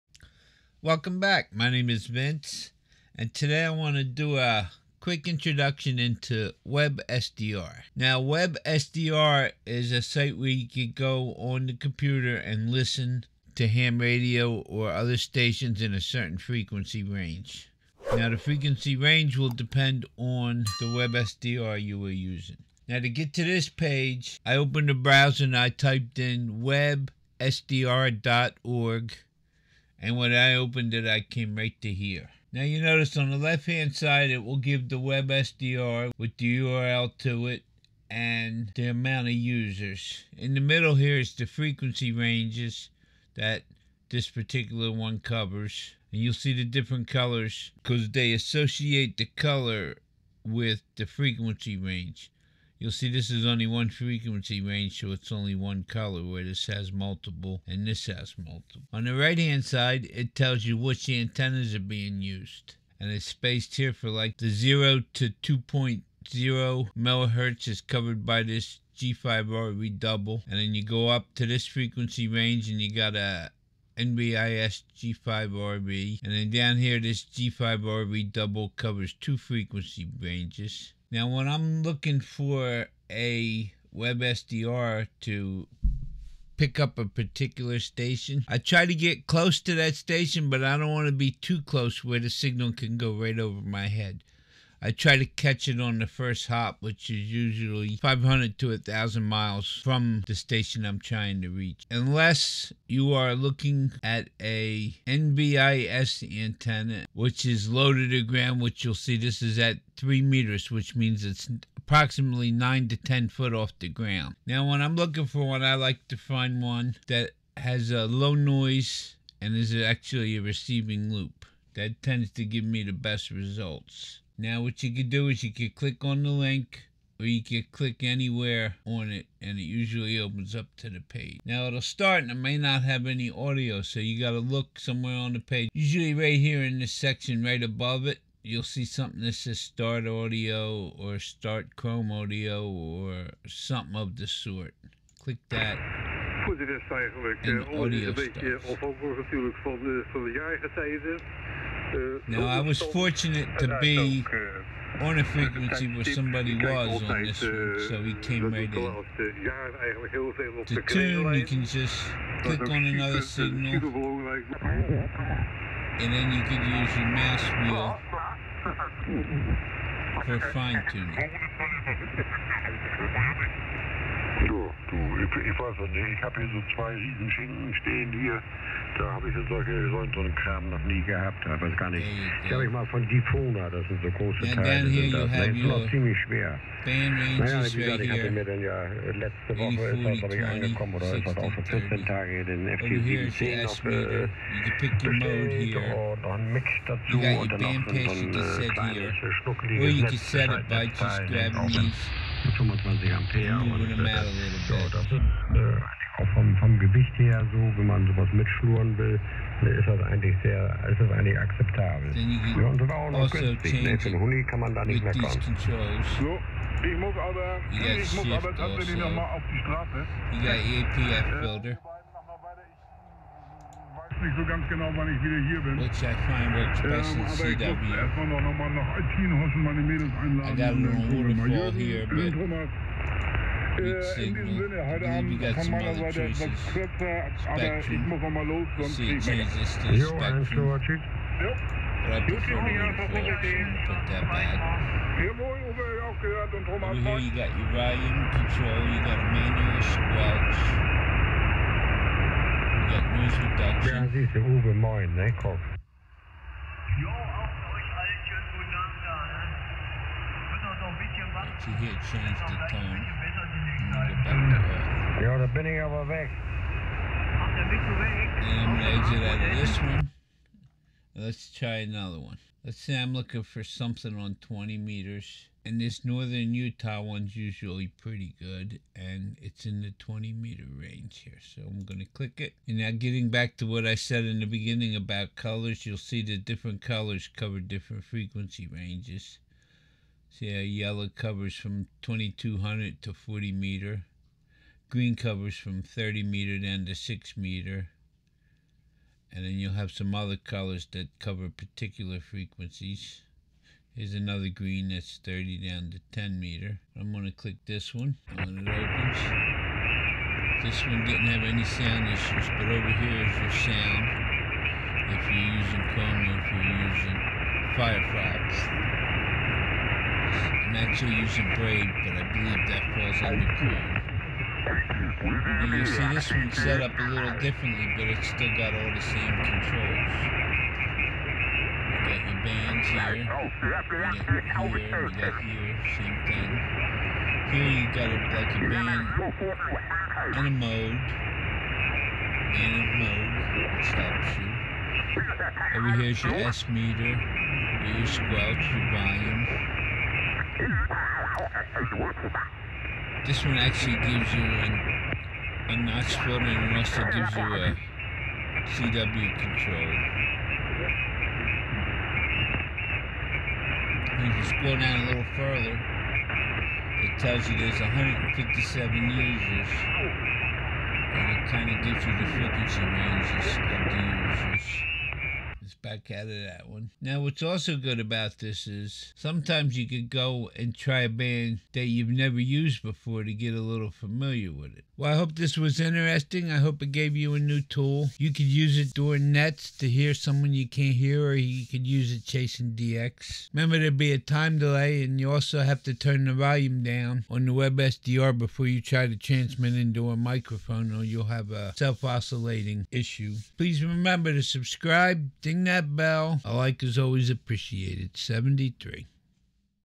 HOW TO USE WEBSDR / GETTING TO KNOW WEBSDR / WEBSDR.ORG
How to use websdr as a powerful online platform that revolutionizes the way we receive amateur radio signals.

In this video, getting to know websdr software capabilities and a demonstration of how WebSDR.org harnesses this technology to provide users with access to a vast array of radio receivers located around the globe.

Follow along as we explore the intuitive interface of WebSDR.org, showcasing its user-friendly features and functionalities. From tuning into local FM stations to decoding digital signals and monitoring shortwave transmissions from distant countries, the possibilities are endless with WebSDR.org.

Discover how WebSDR.org empowers users to delve into different frequency bands, ranging from longwave and medium wave to shortwave and beyond. Experience the thrill of real-time spectrum analysis and gain insights into the invisible world of radio waves.

My gear
Camera Canon
Rode Camera Microphone
Rotating Table
Green Screen